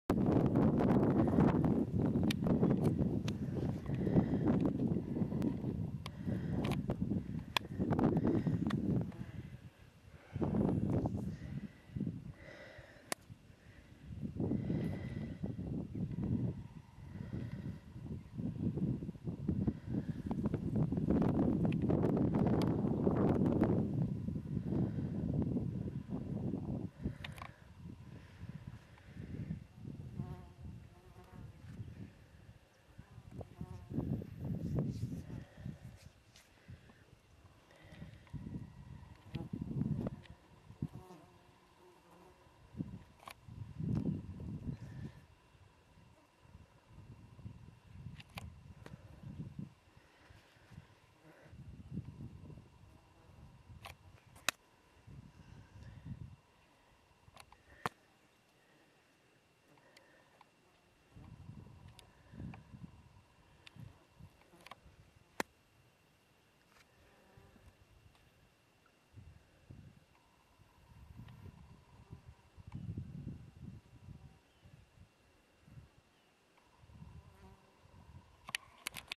Baltimore Checkerspot
Only the second time I have found these wanderers on Crex Meadows.  Filmed July 6, 2011